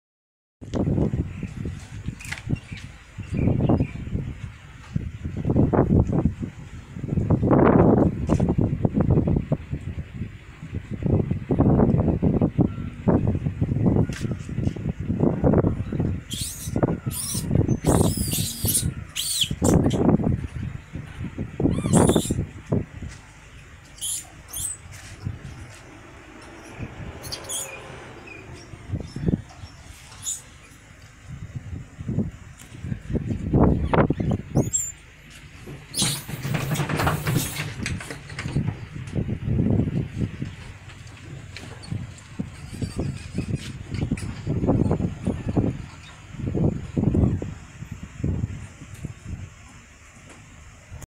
ลิงที่เขาสามมุข
เขาสามมุข จุดชมวิวบางแสนที่มีลิง ที่ท่องเที่ยวของจังหวัดชลบุรี